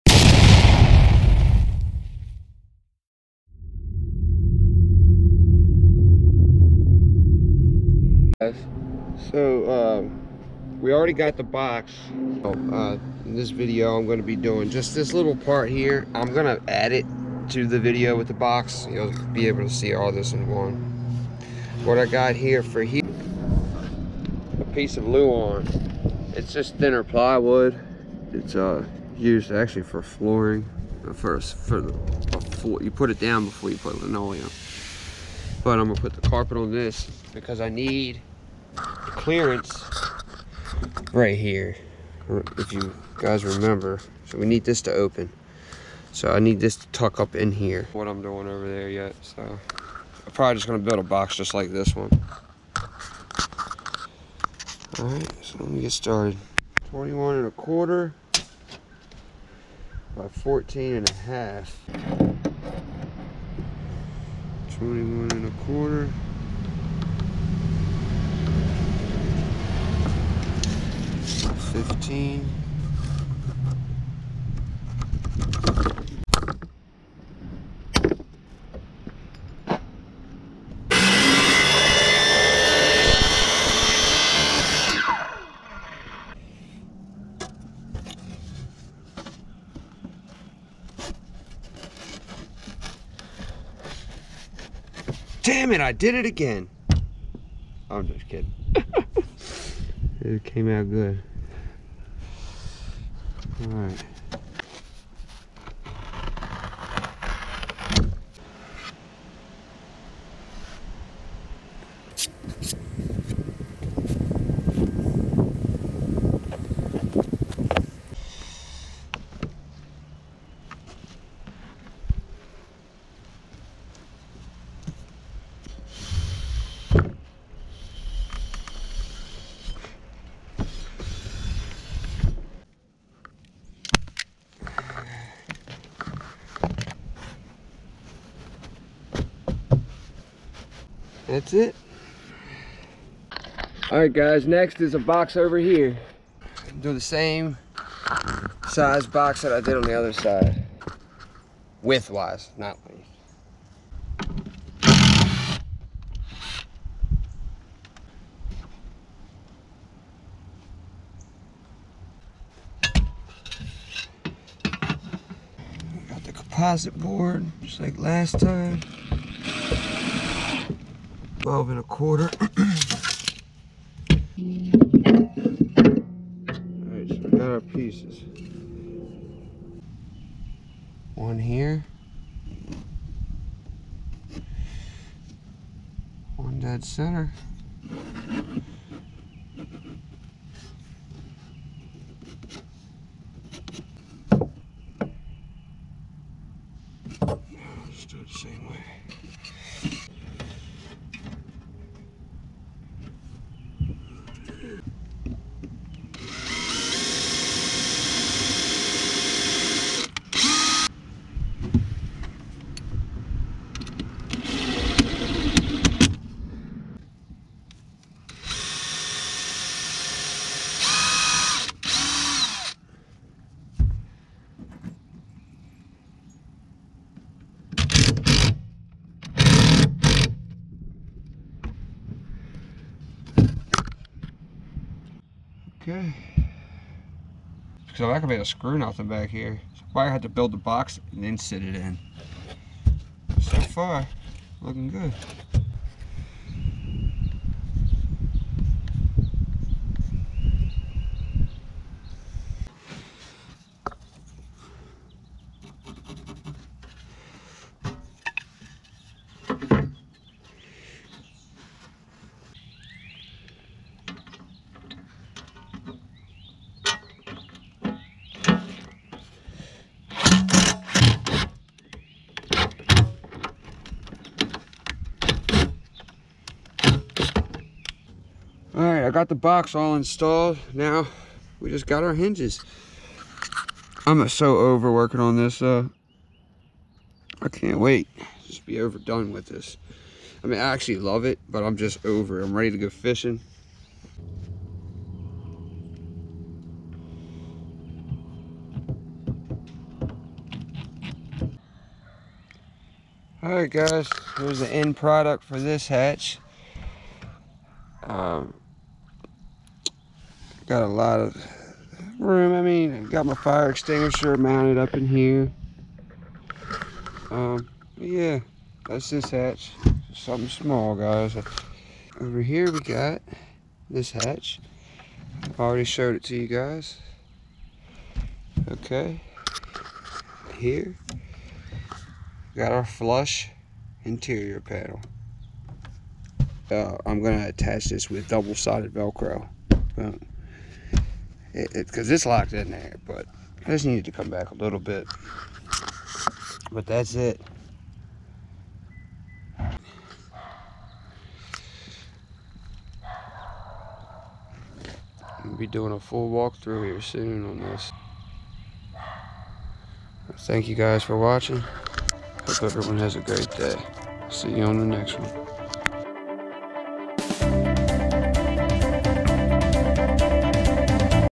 Part 2 is here! Interior carpet with side hatches on jon boat. REDEyE jon boat.JordanMichael Fishing
Hey guys so here is part two of the interior carpet on the floor along with side hatches. It turned out awesome in my eyes check it out. The extra storage always helps also . There cant ever be too much storage.! JordanMichael fishing